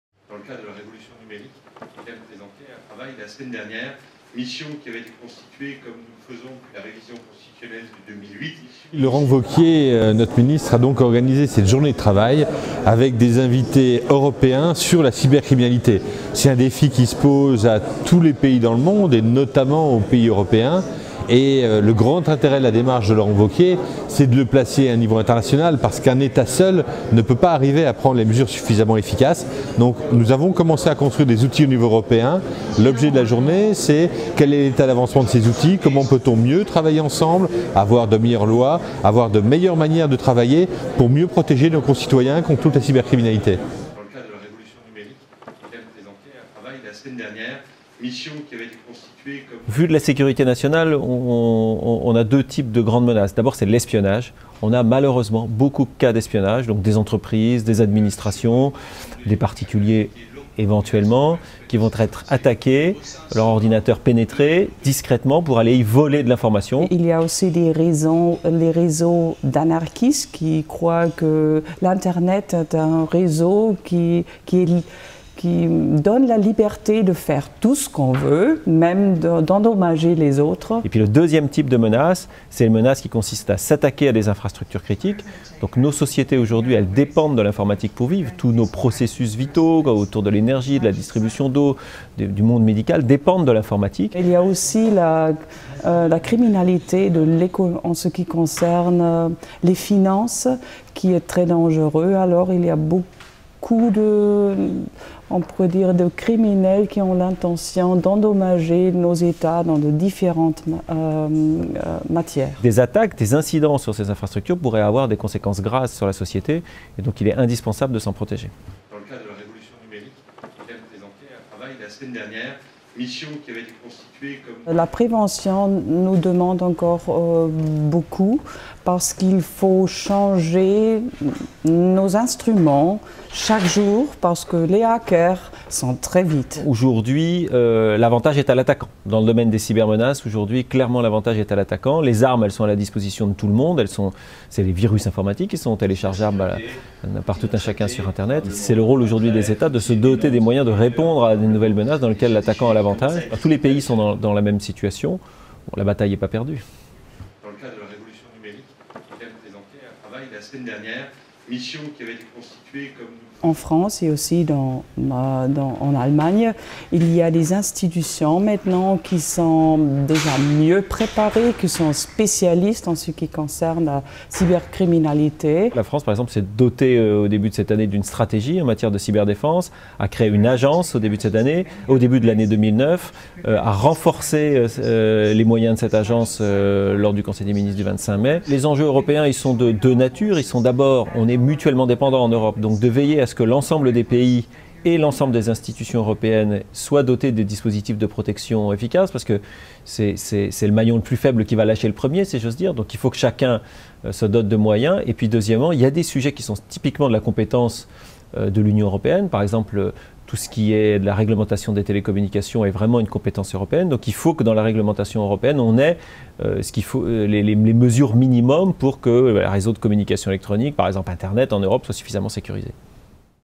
Colloque "L'Europe face aux cybermenaces" - 28 juin 2011
Le ministère a organisé un colloque le 28 juin un colloque sur les moyens que l'Union européenne peut mettre en œuvre pour faire face à la cybercriminalité.

Instragram : @francediplo